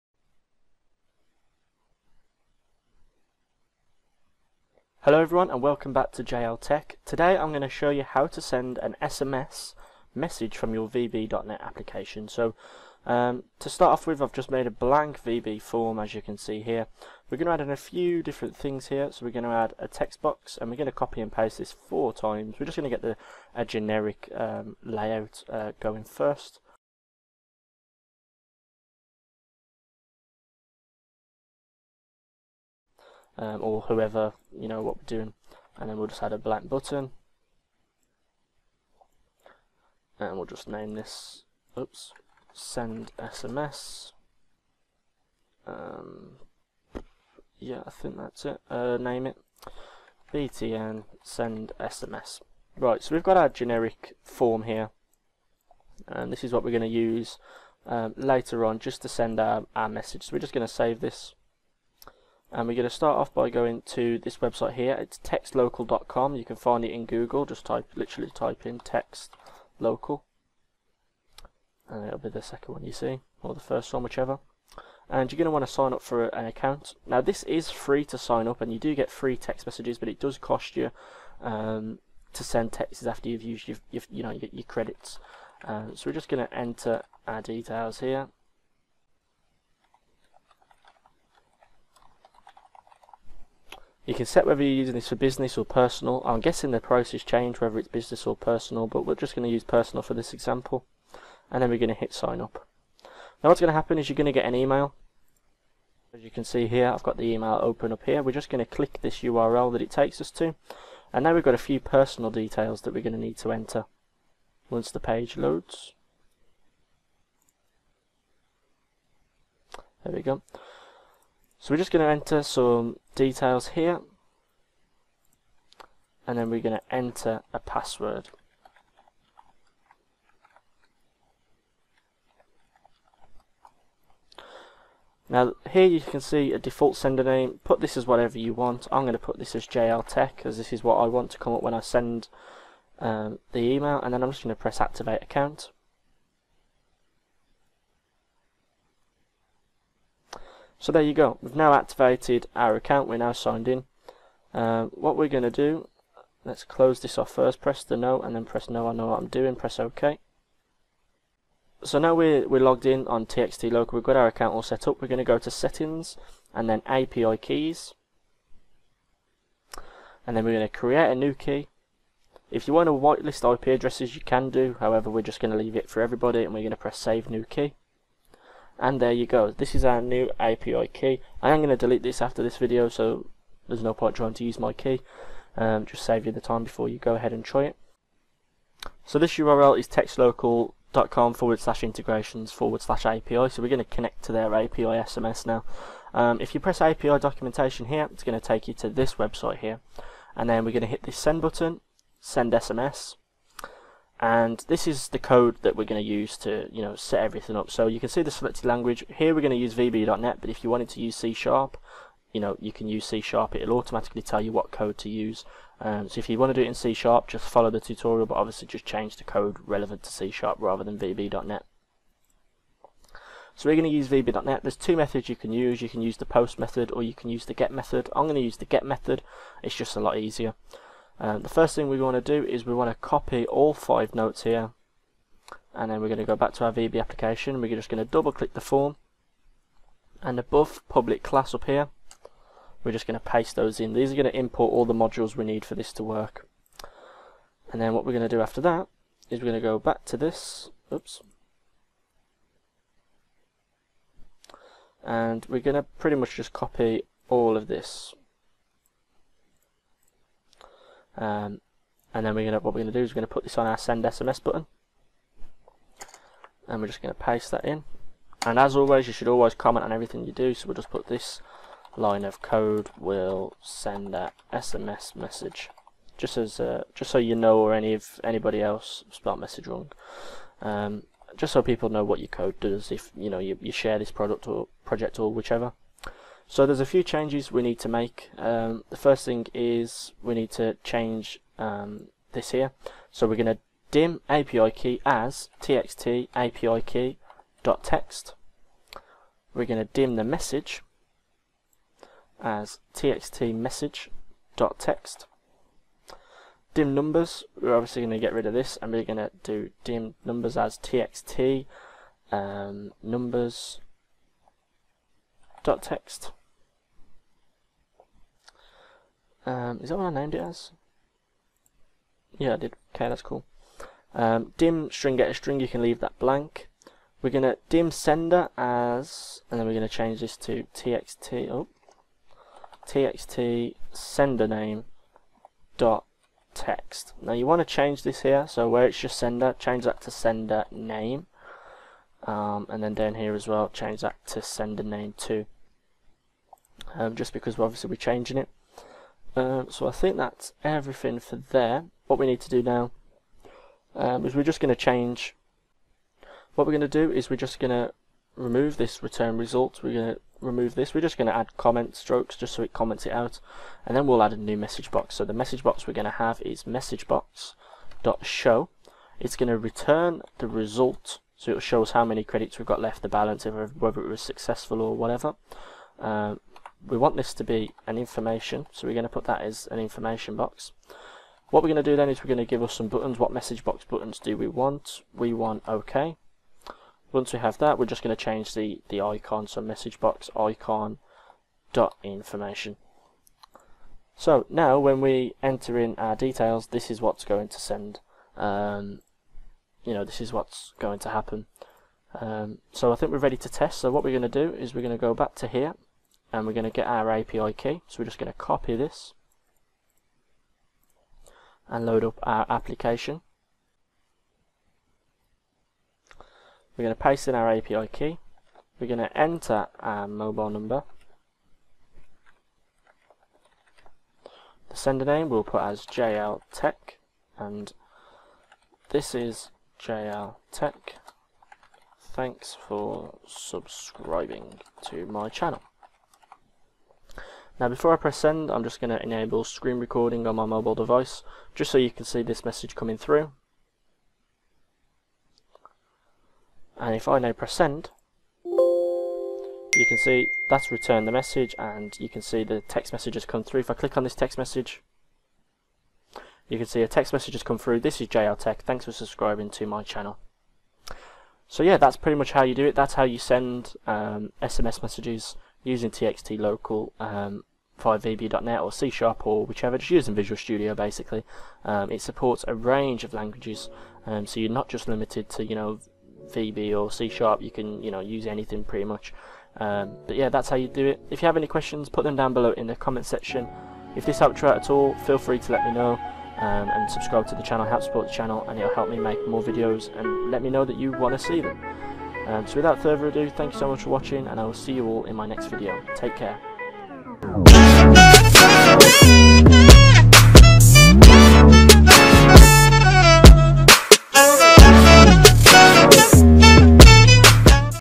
How To Send an SMS Message Using VB.NET OR C#
In this video, I show you how to send an SMS message using VB.NET OR C#

This tutorial demonstrates signing up for TXT Local, Setting up your application, and finally sending/receiving an SMS message.

If you get stuck on this or get any errors, feel free to DM me!

Timestamps:

0:00 - Introduction
0:28 - Building The Layout In Visual Studio
1:15 - Signing up for TextLocal
2:40 - Creating an API Key
3:00 - Viewing API Documentation
4:00 - Starting To Code
6:51 - Testing Application
8:17 - Outro